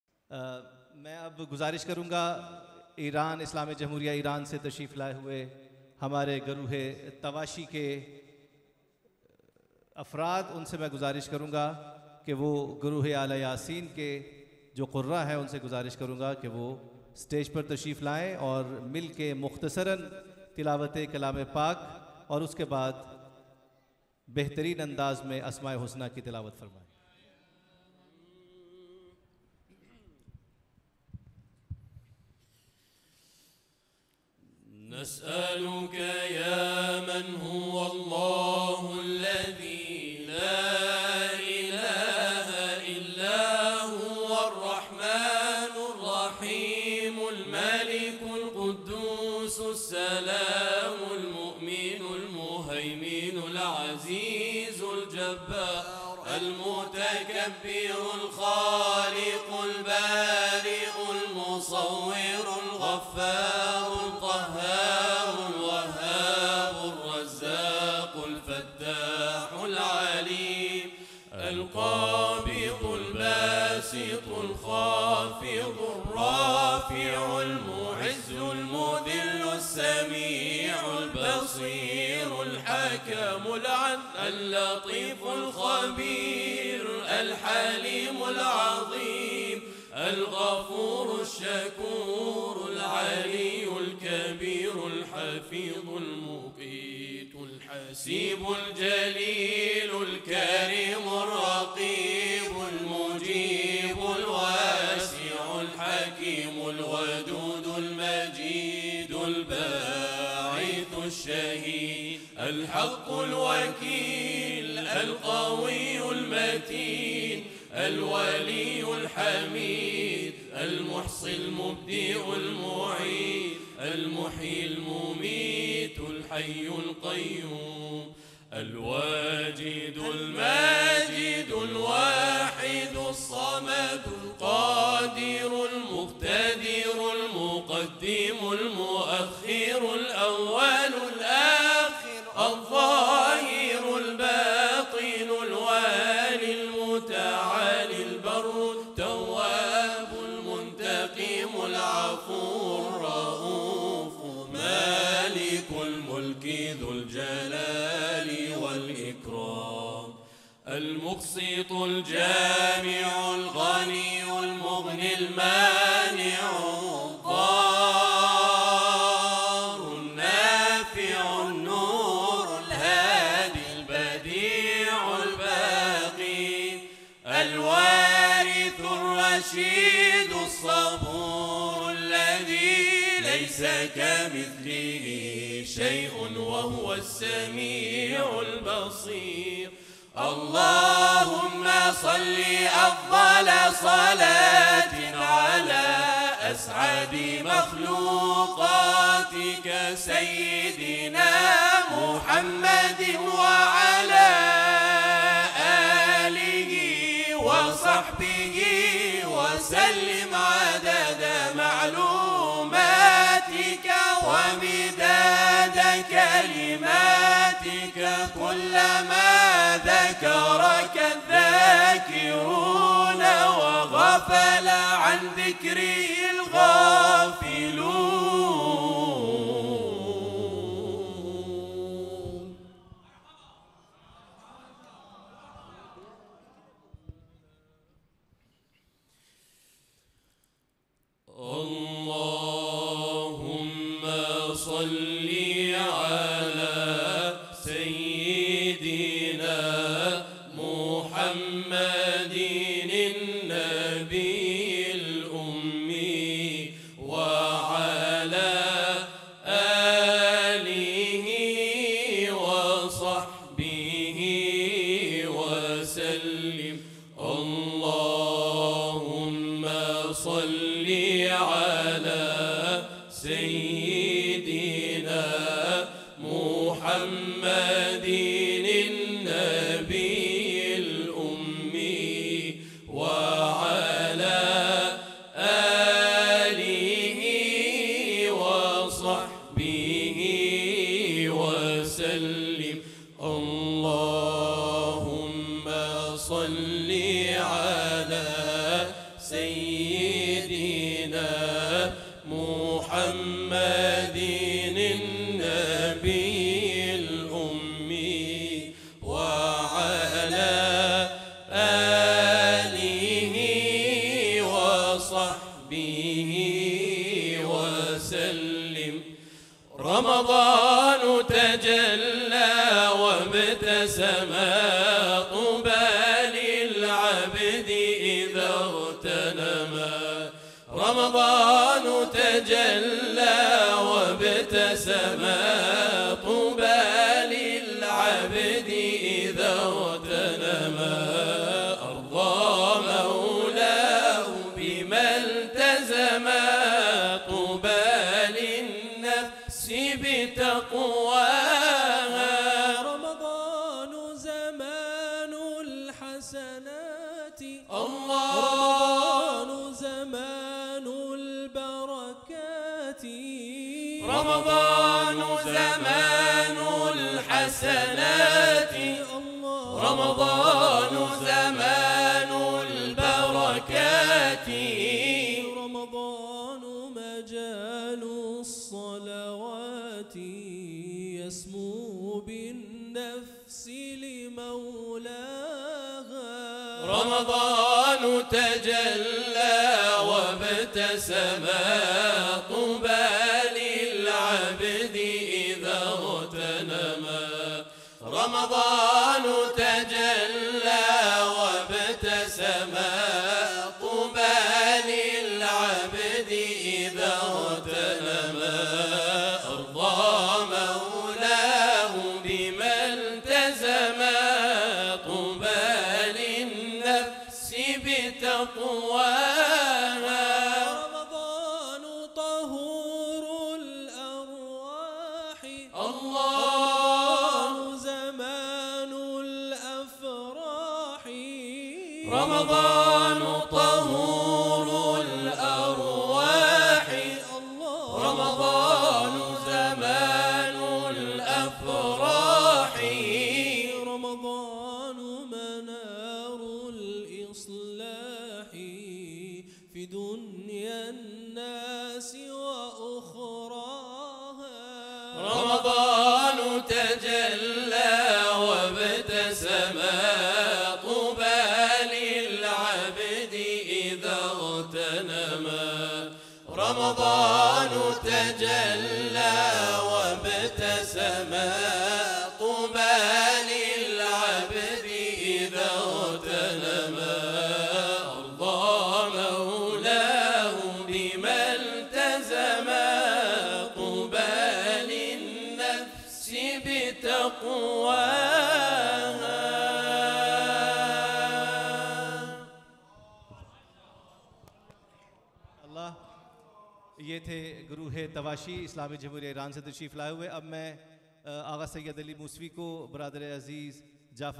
Ale Yaseen Group (IRAN) || International Mehfil e Nozool e Quran 2024
International Mehfil e Nozool e Quran 2024
Reciter              Ale Yaseen Group (IRAN)
Mejanib       Imamia Qirat College Lahore
Recoding by @AUONProduction